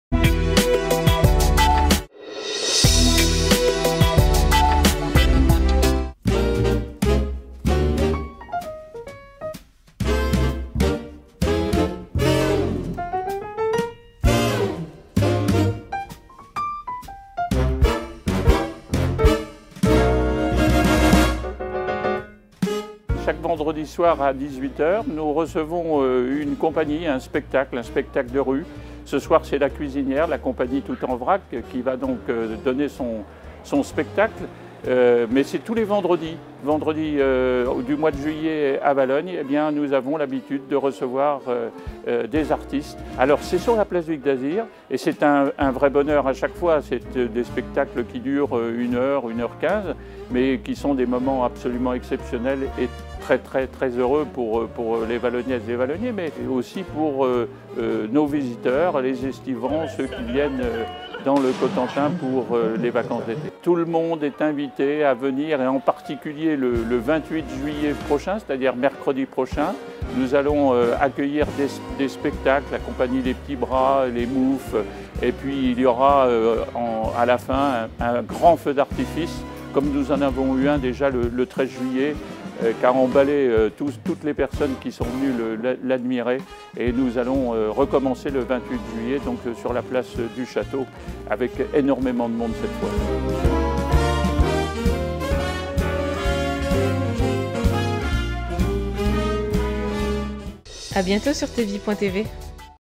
"LES ESTIVALES" VALOGNES (50)
"Les Estivales" est une série de spectacles de rue, gratuits, donnée à Valognes, en Cotentin, en Normandie.

Hôtel-Dieu - Service Culturel de la Ville de Valognes
Tout En Vrac